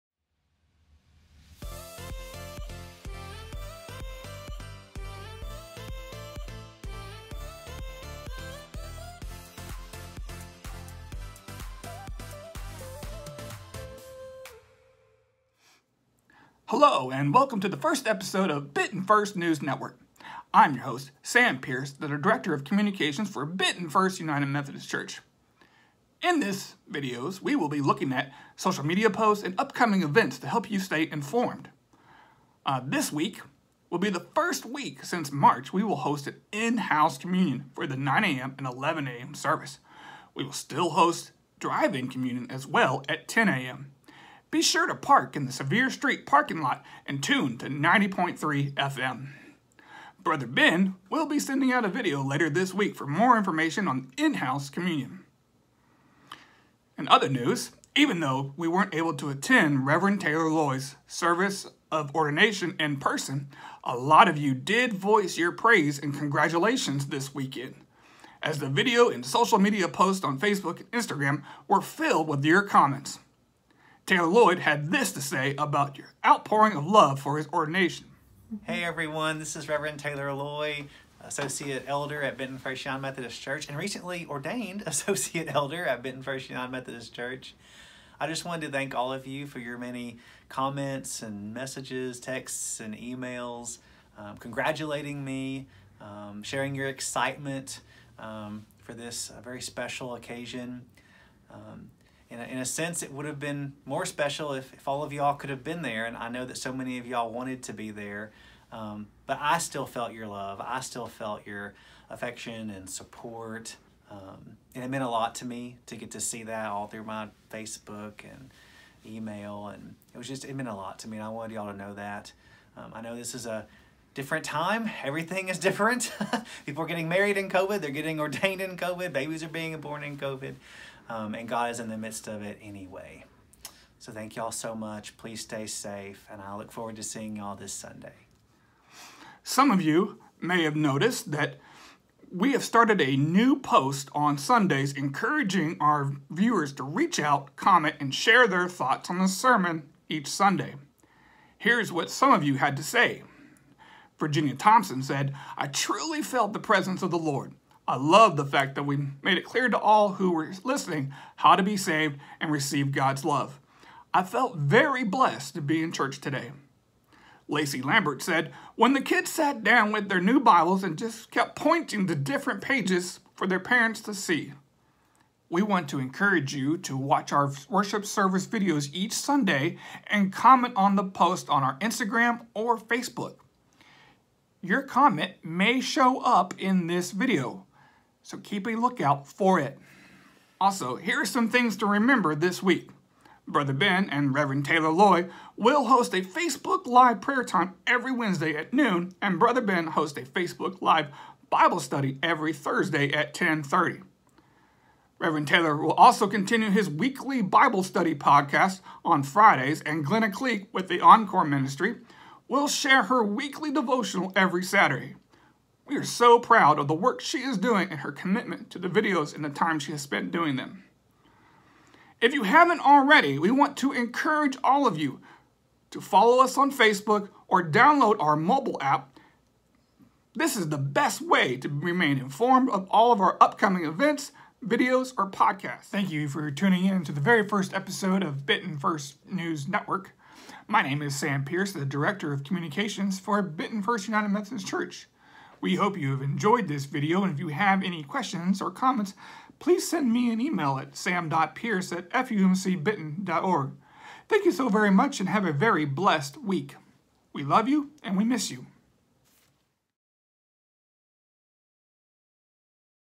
Benton First News Network
Sam Pierce, the director of communications for Benton First United Methodist Church, is the host of the new media series, Benton First News Network.

Each week, he will update you on upcoming events, social media posts, podcasts or other things of interest. This week, he discusses the outpouring of support from the congregation for Rev. Taylor's service of ordination on Sunday.